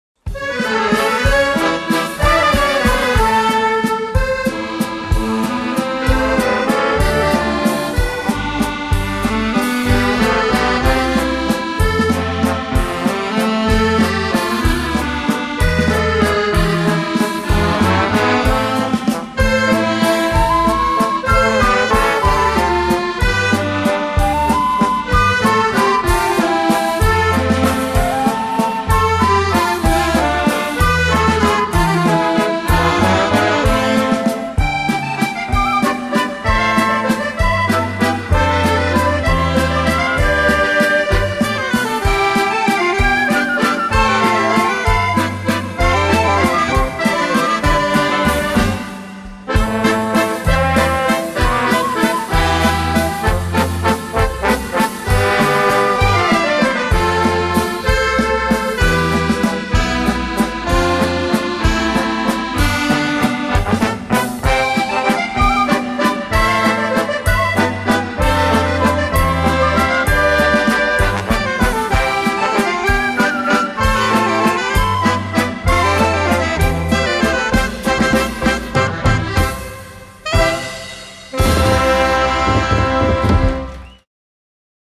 IV Trofeo Internacional de Baile Combinado de Almuñecar Vals +60c
IV Trofeo Internacional de Baile Combinado de Vals, Tango y Pasodoble Ciudad de Almuñécar
IV Trofeo Internacional de Chachachá Ciudad de Almuñécar
IV Trofeo Internacional de Salsa Ciudad  de Almuñécar.